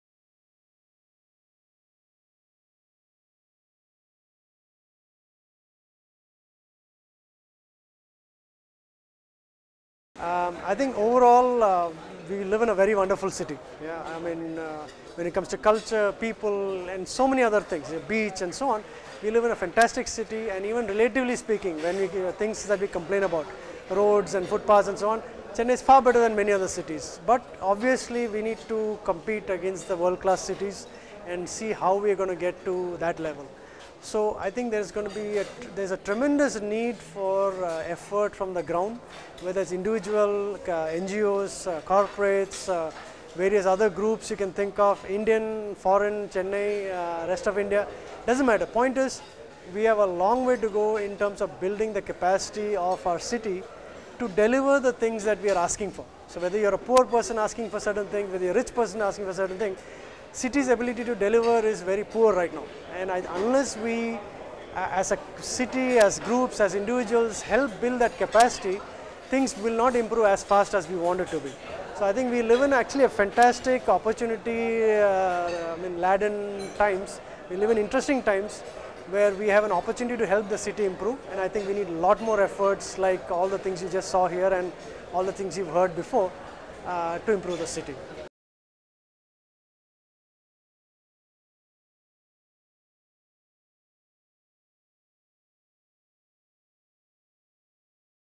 BYC Opinion leaders - Raj Cherubal
Raj Cherubal, Director -- Projects, Chennai City Connect

Raj Cherubal is the Director-Projects for Chennai City Connect (CCC).  CCC, a platform for industry associations and civic organisations, envisions vibrant Indian cities that deliver world-class quality of life, infrastructure and services for all its citizens—rich and poor.

Raj is very much interested in improving urban governance, urban quality of life and promoting market-based solutions to poverty. At CCC he has initiated and/or participated in various projects on traffic, transportation and urban planning focused on pedestrians, Bus Rapid Transit (BRT) and integrated public transportation, efficiency of road system, redevelopment of business district and regional planning.

Raj has written and worked to promote decentralisation and good urban governance. As part of Janaagraha Centre for Citizenship and Democracy (JCCD) Raj and team promoted concepts like effective, decentralised urban centres, Mayoral system of governance, and Area Sabhas to promote meaningful public participation.

Earlier Raj Cherubal was Vice President at Centre for Civil Society (CCS) where he was part of the School Choice Campaign to empower poor parent with resources to make their own decisions regarding their children's education. Part of a growing global movement to promote school choice, they have met thousands of Dalit and tribal parents, among others, to initiate dialogue on expansion of supply of good quality schools for their children.

He also focused on promoting economic freedom for entrepreneurs, especially poor like street vendors and others in the informal sector and for their recognition as profit seeking entrepreneurs. 70% or more of Indian workforce is in the informal sector. This sector still languishes under the dreaded license-permit raj, causing incalculable damage to livelihood and immense potential for wealth creation and poverty elimination.